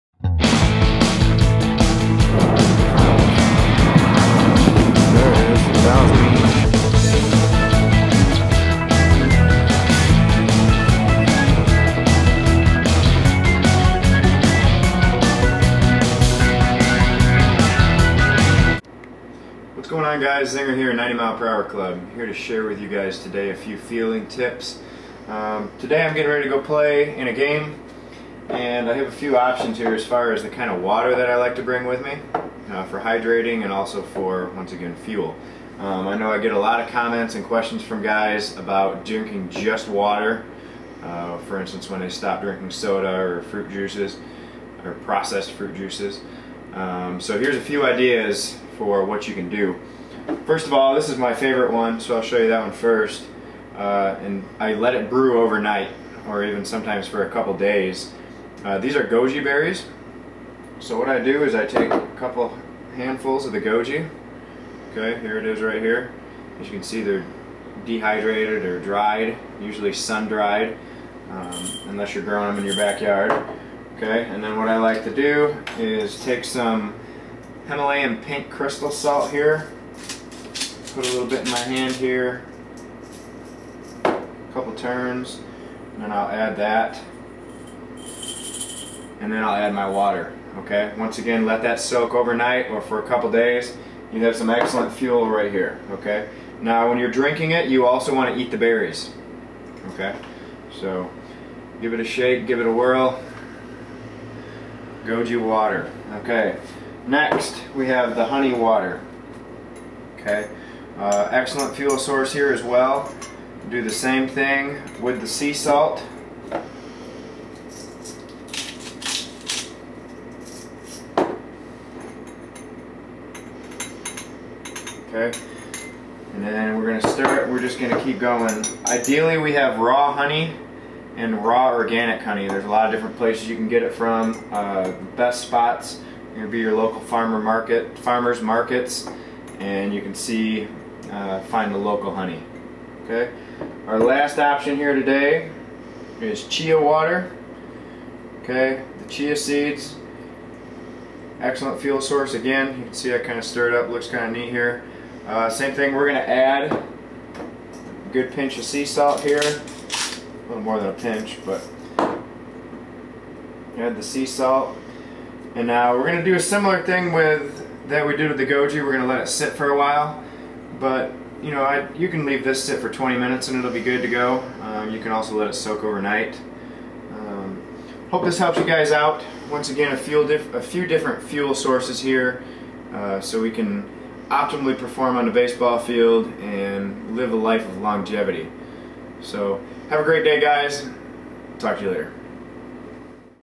Goji, Chia, and Honey Water
90 MPH Club Fueling

Today Zinger shows you a variety of waters you can add to your fueling plan.

Goji Water

Chia Water

Honey Water

Get out and move today guys.  Make sure you are using your 90 MPH Club and Zinger Workouts DVDs as a reference, but get out do some freestyle moving.

Have a great day and thanks for breathing!